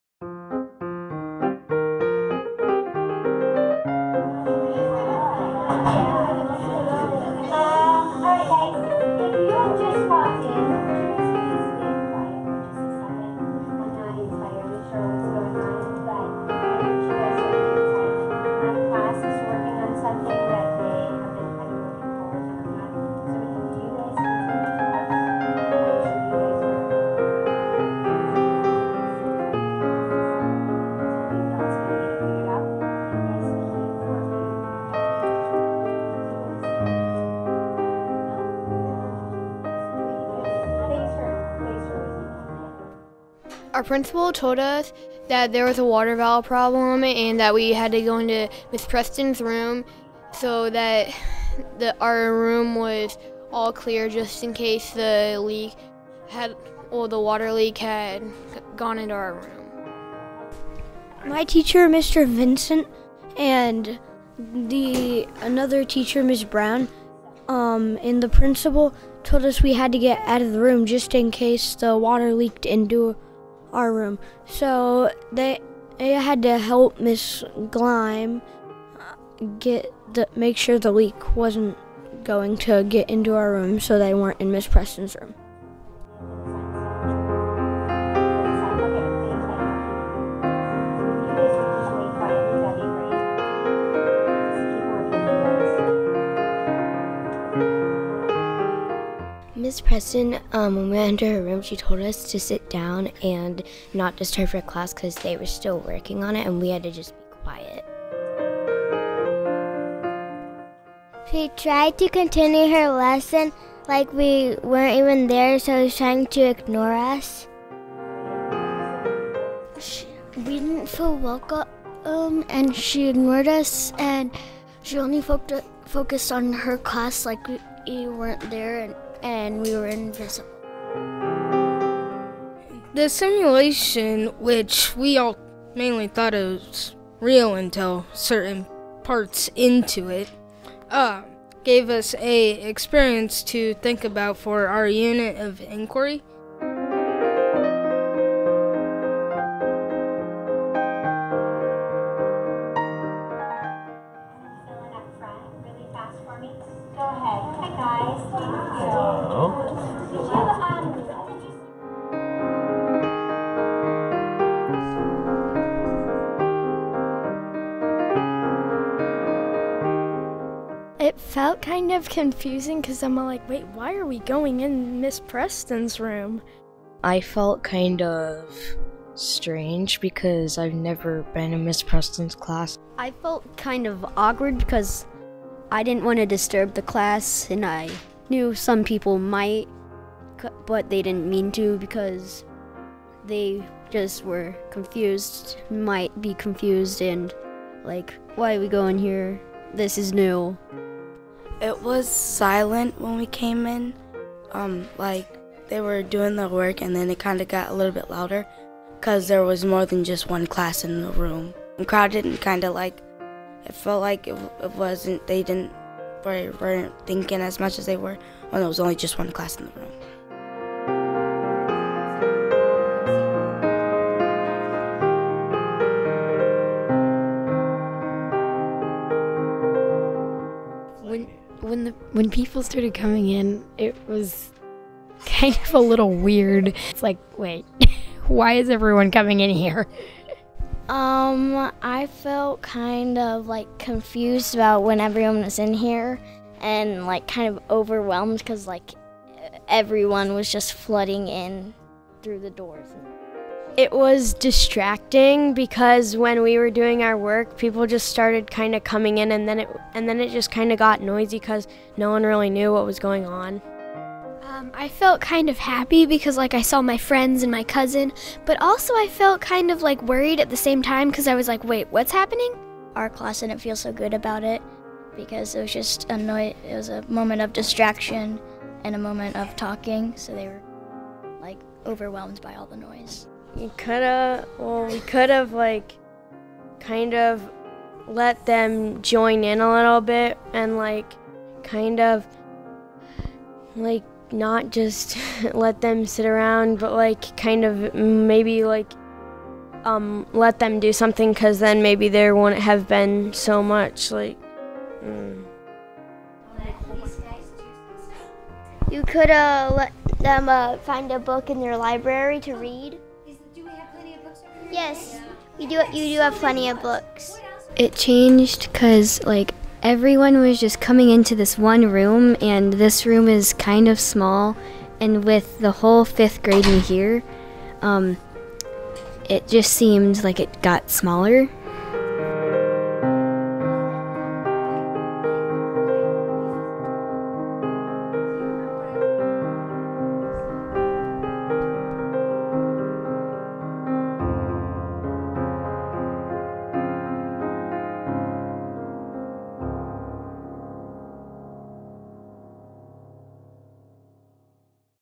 🔀 Immigration Simulation
Fifth graders at College View Elementary School participated in a simulation to spark exploration and discussion about movement, perceptions, immigration, and change.